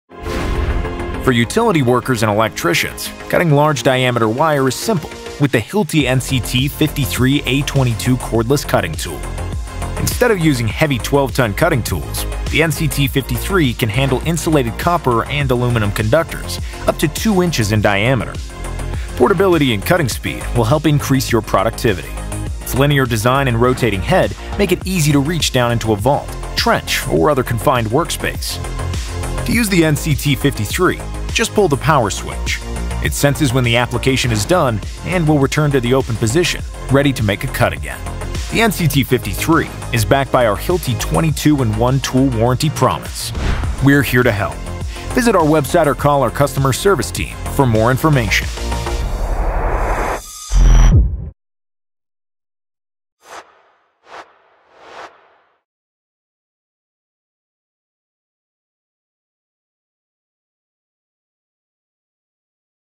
INTRODUCING Hilti Cordless Wire Cutting Tool NCT 53-A
Want to learn more? Canada

The new Hilti NCT 53-A22 cordless CU/AL cutter offers the
ability to cut copper and aluminum conductor up to 2” in
outside diameter, in a lightweight tool. The inline design
allows users to reach into applications that larger 12 ton tools
struggle to fit into. The head swivels 350 degrees to provide
the access and maneuverability needed to get the job done.
The NCT 53-A22 CU/AL cordless cutter is part of the growing
Hilti 22v cordless platform and covered under the industry
leading 20-2-1 warranty.